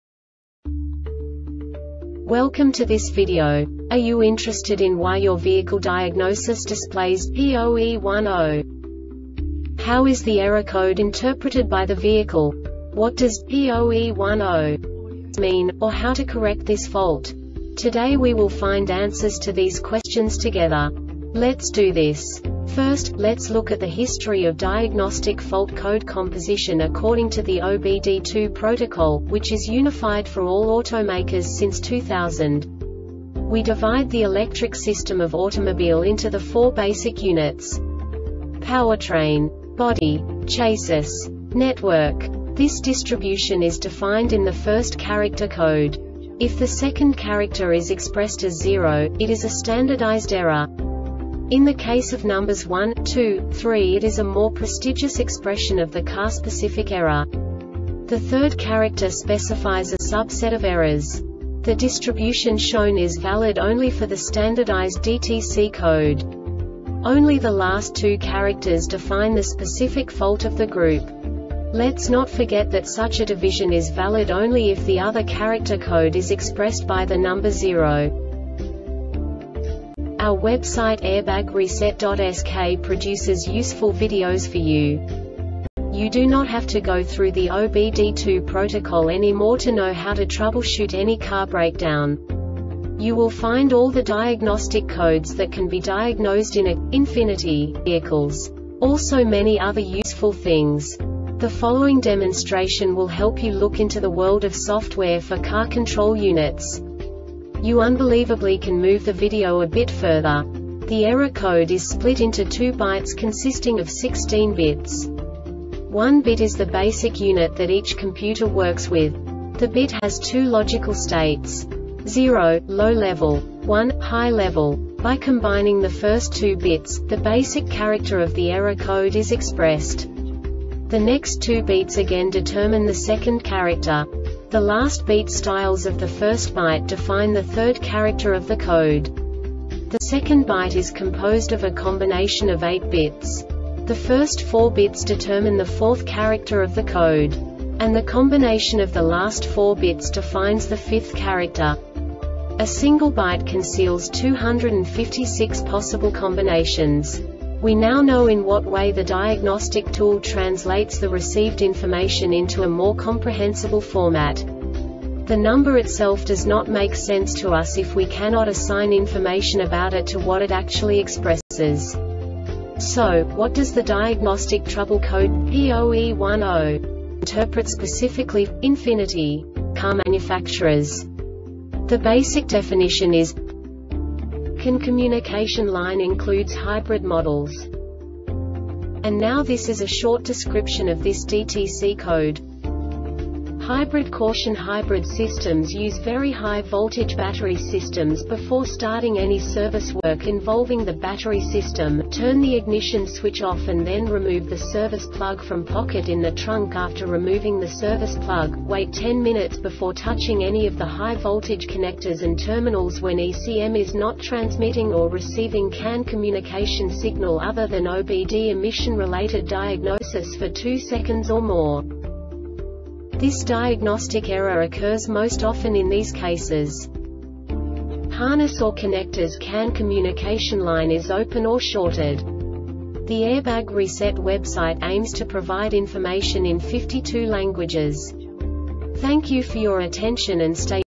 DTC Ford P0E10 Short Explanation
Contents:
0:21 Basic DTC analysis according to OBD2 protocol standard.
1:48 Insight into programming
2:58 A diagnostically understandable description of the Diagnostic Trouble Code
3:05 Basic error definition

Make:
Ford
Code:
P0E10
Definition:
CAN Communication Line (Includes Hybrid models)
Description:
HYBRID CAUTION: Hybrid systems use very high-voltage battery systems.Before starting any service work involving the battery system,turn the ignition switch OFF and then remove the service plug from pocket in the trunk.After removing the service plug,wait 10 minutes before touching any of the high-voltage connectors and terminals.When ECM is not transmitting or receiving CAN communication signal other than OBD (emission related diagnosis) for 2 seconds or more.
Cause:
 Harness or connectors- CAN communication line is open or shorted